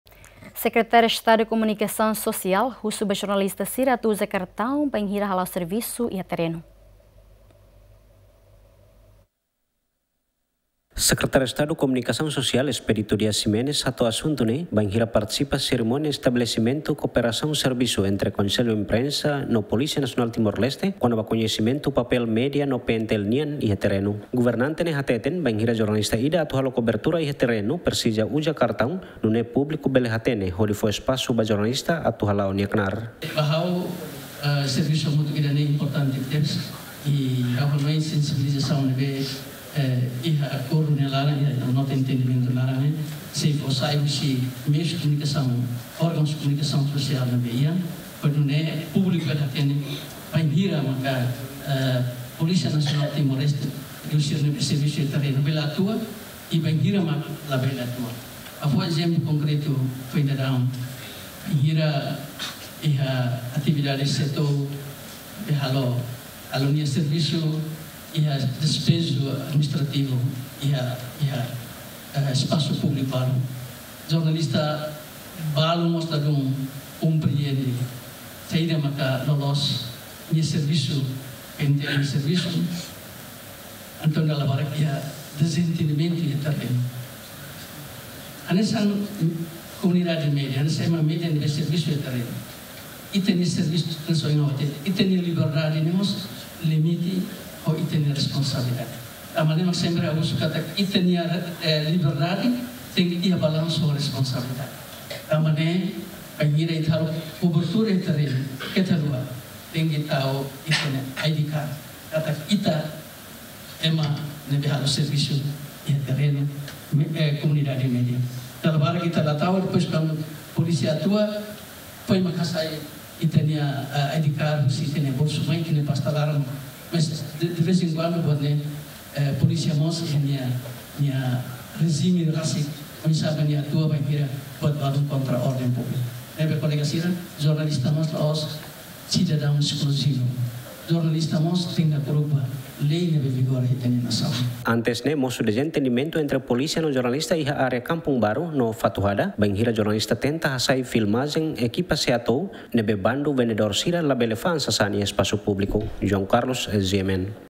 SEKOMS husu ba Jornalista sira atuuza kartaun bainhira halao servisu iha trenu
Sekretariu Estadu Komunikasaun Sosial husu ba Jornalista sira atuuza kartaun bainhira halao servisu iha trenu